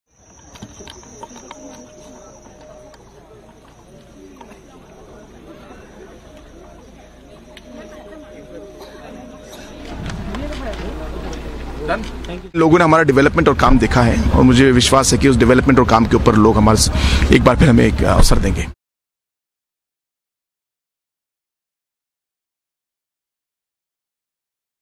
Meghalaya polls: CM Conrad Sangma casts vote, says party is fighting on the issue of development
Voting in Meghalaya for 59 out of the 60 assembly seats are under way. The northeastern state has recorded a voters turnout of over 27% till 11am. The polling will seal the fate of 369 candidates. Polling in Sohiong assembly constituency has been adjourned due to the death of one of the candidates. The ruling National People's Party will fight to retain power, while the BJP and Trinamool Congress will attempt to unseat the incumbent government from power. CM Conrad Sangma, sister Agatha Sangma were present at the voting site in line to cast vote in South Tura.